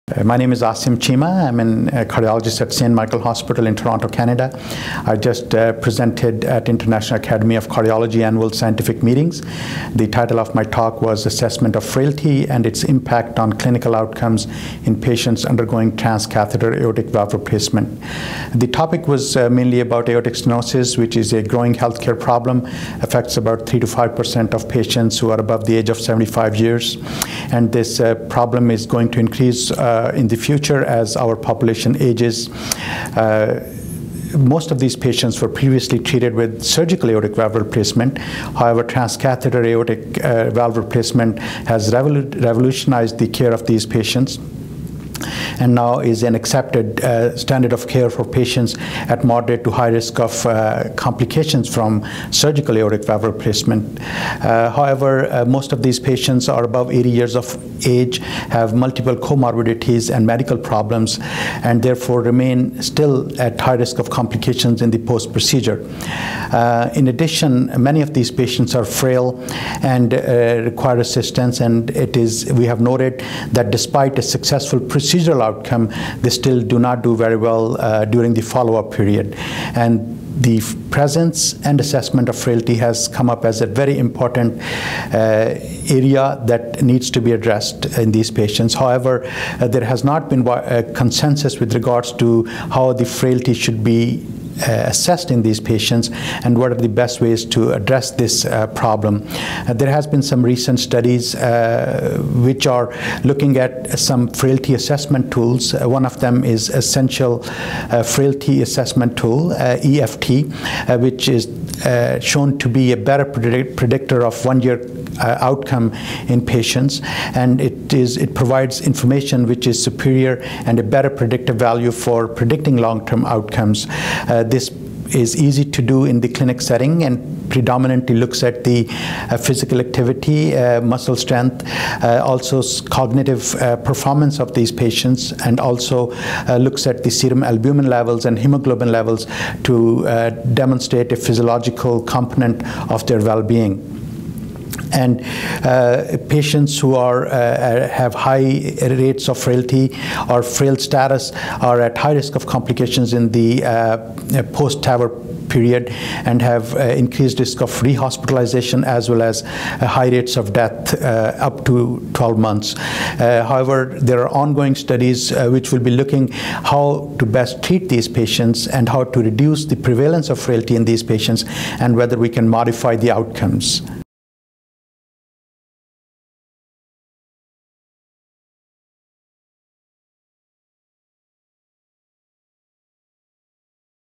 International Academy of Cardiology: Asim N. Cheema, M.D.: FRAILTY AND CLINICAL OUTCOMES OF
FRAILTY AND CLINICAL OUTCOMES OF TRANSCATHETER AORTIC VALVE REPLACEMENT  (Invited Lecture)
Asim N. Cheema, M.D.
St. Michael's Hospital, Toronto, ON, Canada

Presented at the:
23rd World Congress on Heart Disease

PLAN TO ATTEND:
24th World Congress on Heart Disease